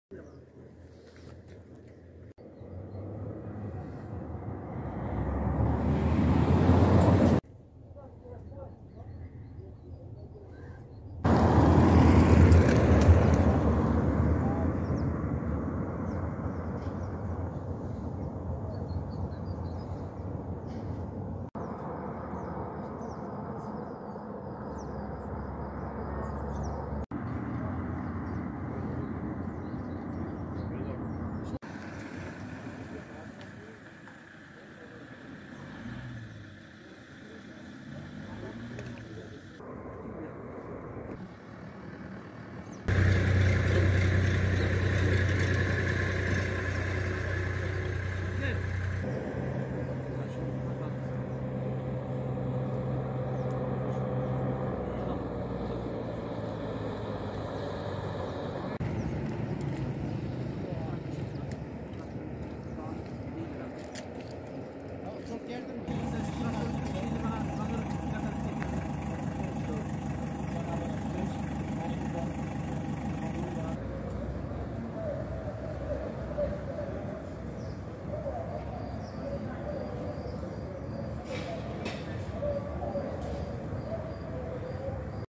İzmir'de traktörle kamyon çarpıştı
İzmir'in Ödemiş ilçesinde kamyonla traktör çarpıştı. Çarpışmada, traktörün sürücüsü hayatını kaybetti, tarım işçilerinin bulunduğu römorktaki 13 kişi yaralandı.